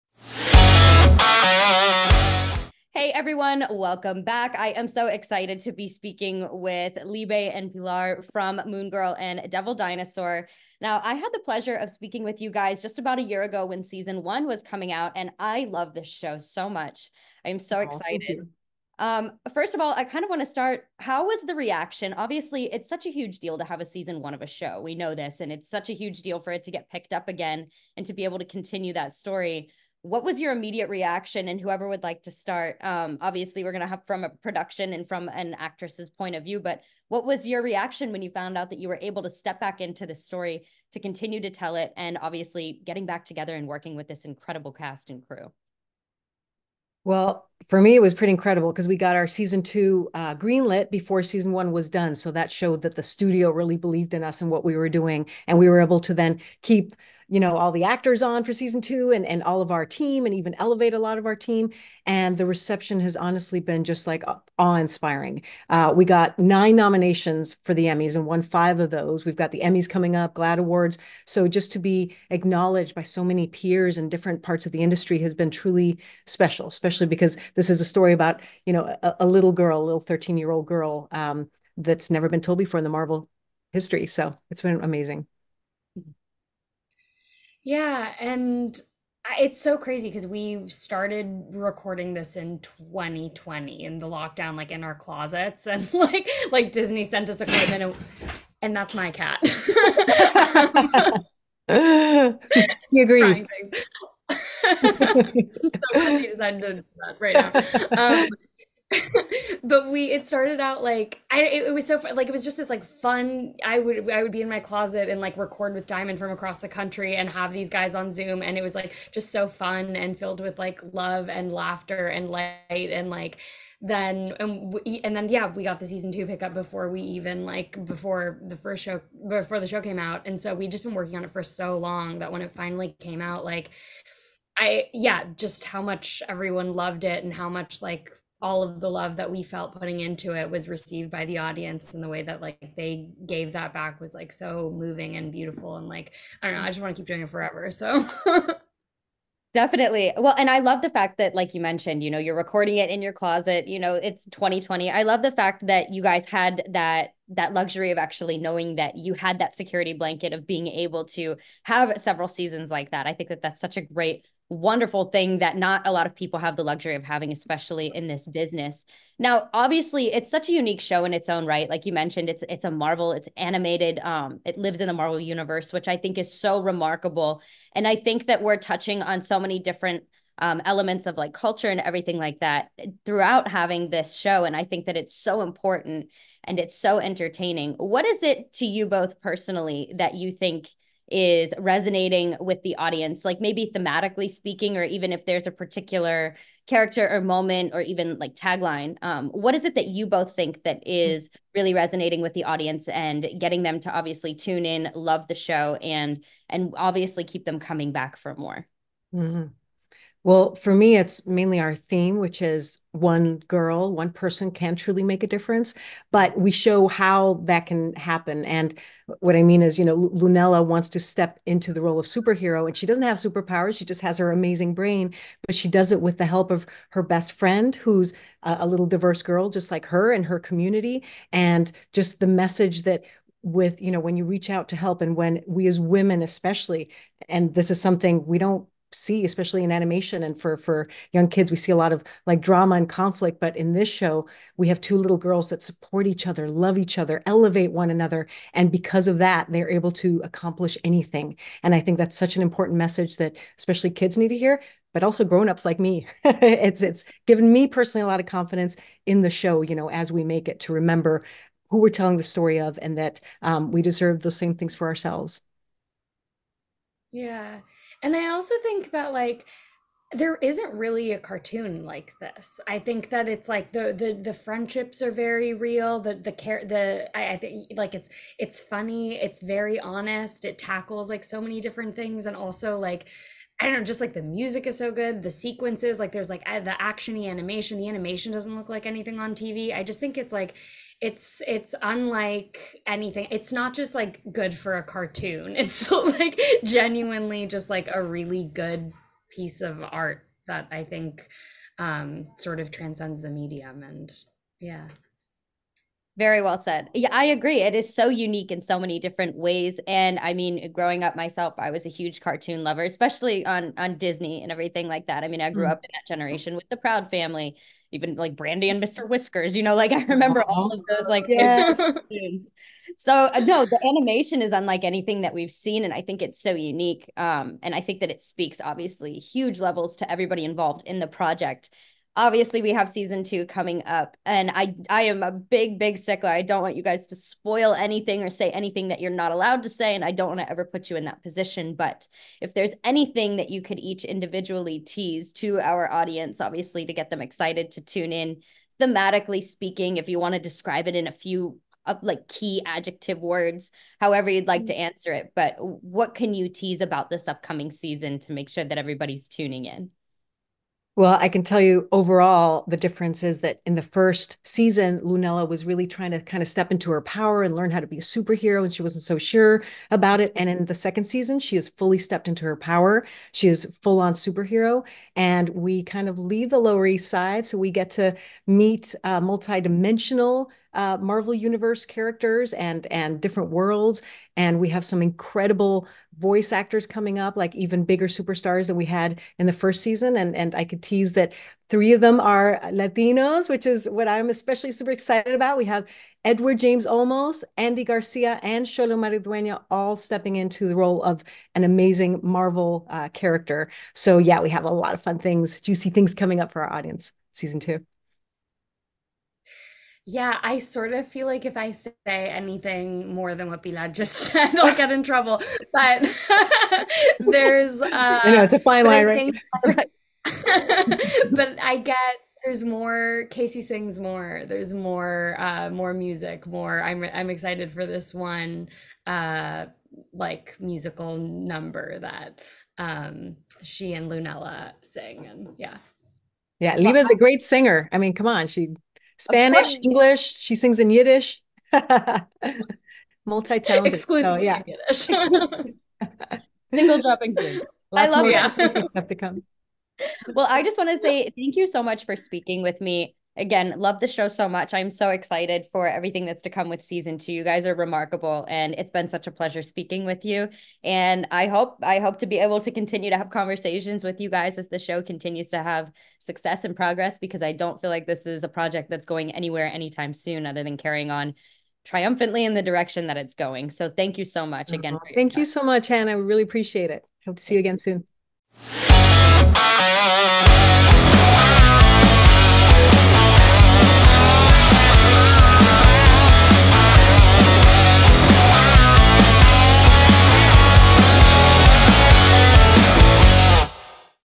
Libe Barer & Pilar Flynn Coming Back For Moon Girl and Devil Dinosaur
NEWS x MUSE Hannah Fletcher talks with Libe Barer and Pilar Flynn on returning for the second season of Marvel Moon Girl and Devil Dinosaur on Disney Channel.

"Marvel's Moon Girl and Devil Dinosaur", based on Marvel's hit comic books, the series follows the adventures of 13-year-old super-genius Lunella Lafayette and her 10-ton T-Rex, Devil Dinosaur, as they protect her Lower East Side neighborhood from danger. In season two, Moon Girl and Devil Dinosaur are in their element as Super Heroes; however, as Moon Girl's Super Hero-ing intertwines more with her personal life, she must decide if the cost of keeping her identity a secret is worth the toll it takes on her family.

Host:
Hannah Fletcher